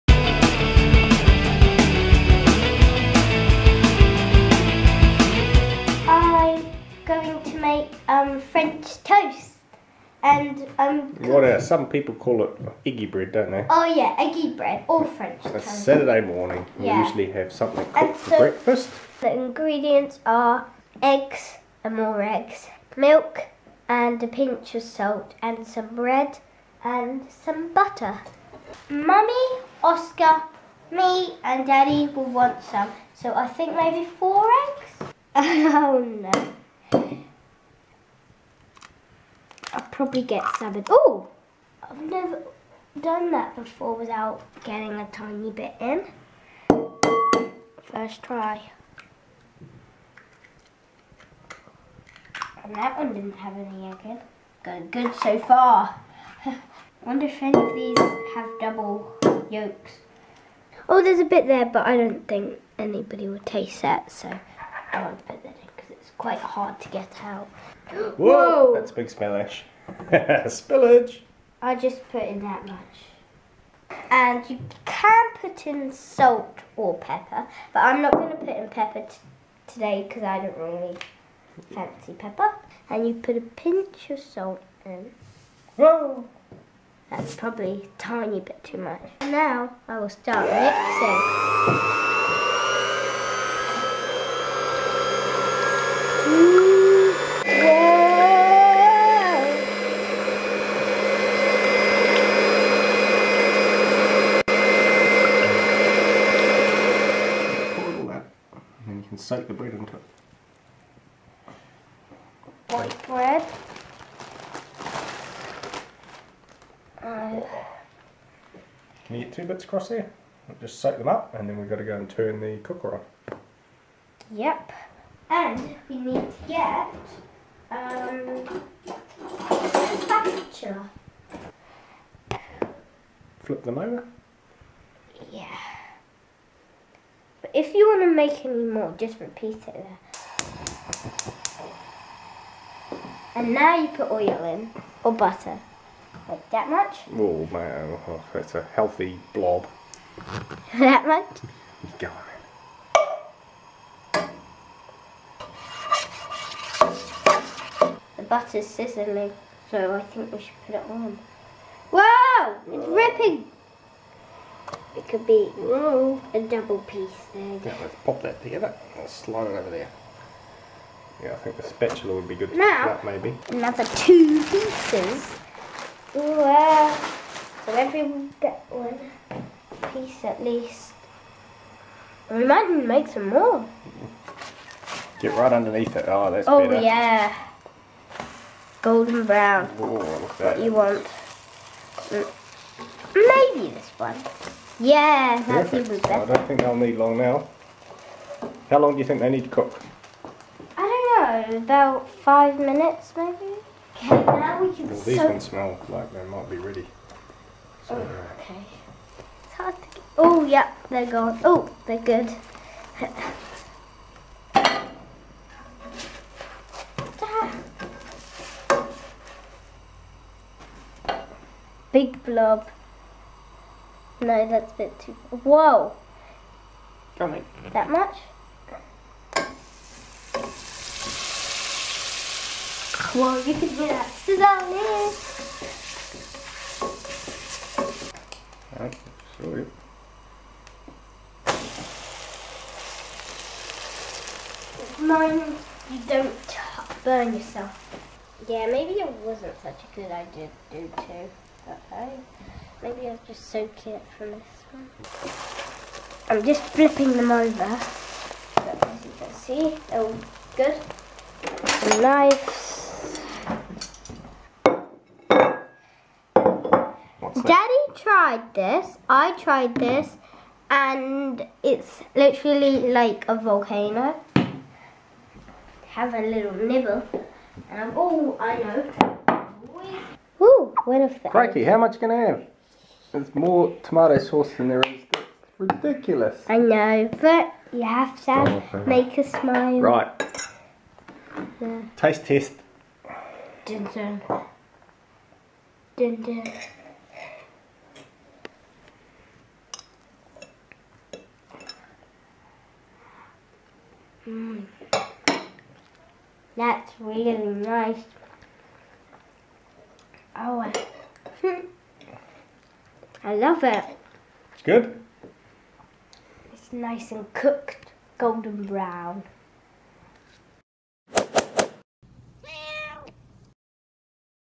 French toast - great kids recipe I twoplustwocrew
Great easy recipe for children, french toast! (eggy bread)
Tabitha cooks Saturday morning breakfast.
We try and get both of our children to cook and french toast is a nice simple recipe to get them started, quick and tasty. plus you get to use the electric whisk, messy the first few times!